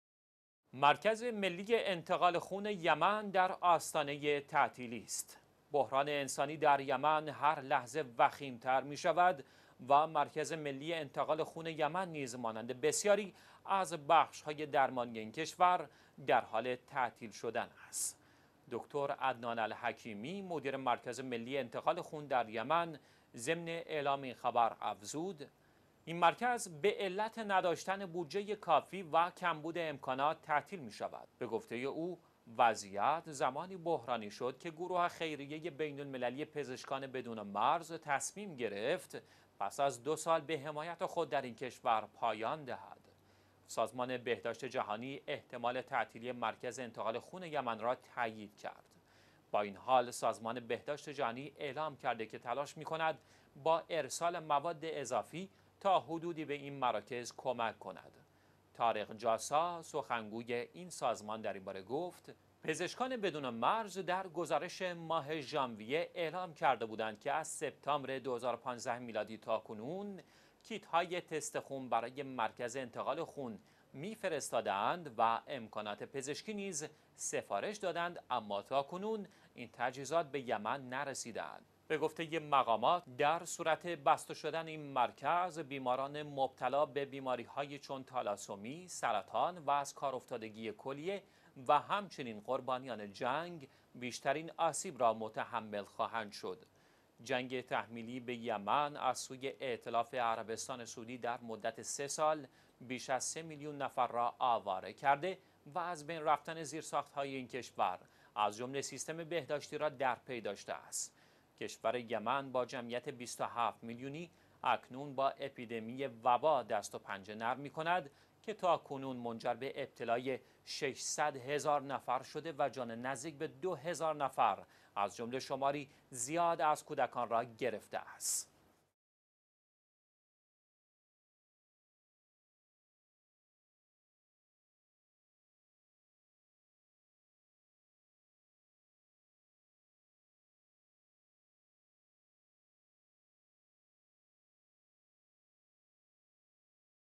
مرکز انتقال خون یمن، در آستانه تعطیلی
مرکز ملی انتقال_خون یمن در آستانه تعطیلی است.

بحران انسانی در یمن هر لحظه وخیم‌تر شده و مرکز ملی انتقال خون یمن نیز مانند بسیاری از بخش های درمانی این کشور،در حال تعطیل شدن است.

«دکتر عدنان الحکیمی» مدیر مرکز ملی انتقال خون در یمن ضمن اعلام این خبر افزود که این مرکز به علت نداشتن بودجه کافی و کمبود امکانات تعطیل می‌شود.

به گفته وی وضعیت زمانی بحرانی شد که گروه خیریه بین المللی پزشکان بدون مرز تصمیم گرفت پس از دو سال به حمایت خود در این کشور پایان دهد.

با این حال، سازمان بهداشت جهانی اعلام کرده که تلاش می‌کند با ارسال مواد اضافی تا حدودی کمک کند.

از سوی دیگر سازمان بهداشت جهانی احتمال تعطیلی مرکز انتقال خون یمن را تایید کرد.

«طارق جاساریویچ» سخنگوی این سازمان در این باره گفت در گزارش ماه ژانویه پزشکان بدون مرز اعلام کرده بودند که از سپتامبر 2015 تاکنون کیت‌های تست خون برای مرکز انتقال خون می‌فرستاده‌ا‌ند، امکانات پزشکی نیز سفارش داده شده‌اند اما تاکنون به یمن نرسیده‌اند.

به گفته مقامات، در صورت بسته شدن این مرکز بیماران مبتلا به بیماری‌هایی چون تالاسمی، سرطان و ازکار افتادگی کلیه و همچنین قربانیان جنگ، بیشترین آسیب را متحمل خواهند شد.

جنگ تحمیلی به یمن از سوی ائتلاف عربستان سعودی تاکنون به 3 سال کشیده شده و بیش از 3 میلیون نفر را آواره نموده و از بین رفتن زیرساخت‌های زیادی در این کشور از جمله سیستم بهداشتی را در پی داشته است.

کشور یمن با جمعیت 27 میلیونی اکنون برای مقابله با اپیدمی «وبا» دست و پنجه نرم می‌کند که تاکنون منجر به ابتلای 600 هزار نفر شده و جان نزدیک به 2 هزار نفر از جمله شمار زیادی از کودکان را گرفته است.